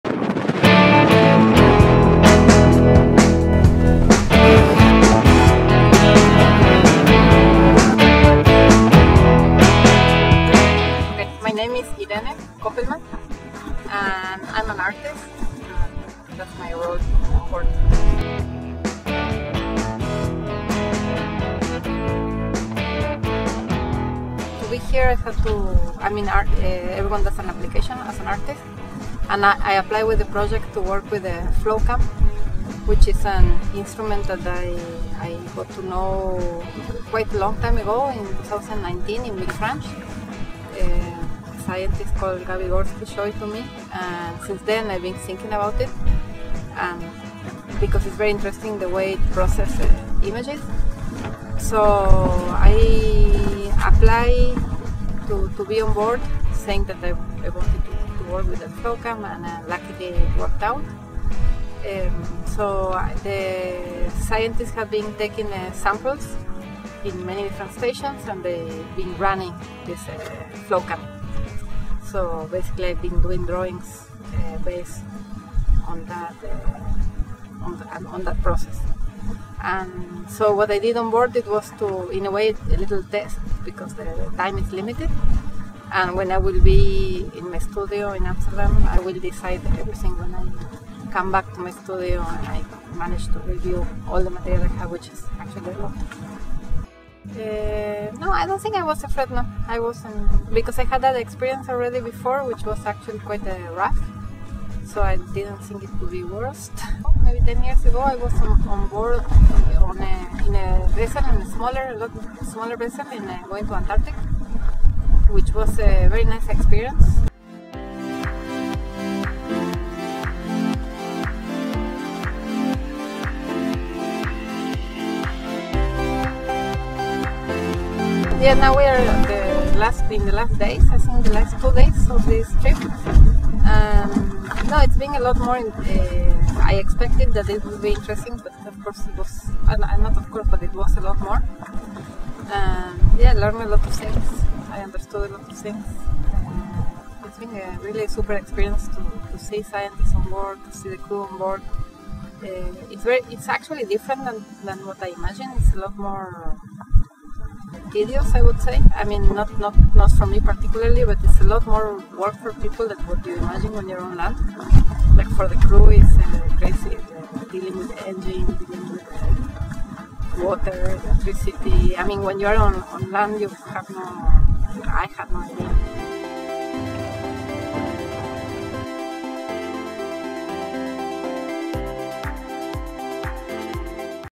Artist in Residence - Irene Kopelman
👩🎨Meeting with Irene Kopelman, artist in residency on Tara between Pointe Noire in Congo and Banjul in Gambia.

🔊 She works with images generated by an instrument called FlowCam, which transforms seawater samples into two-dimensional images through a computer.

She explains her creative journey in this video 👇

To learn more about Irene, discover her portrait on our

🎥 Guillaume Tauran // Tara Ocean Foundation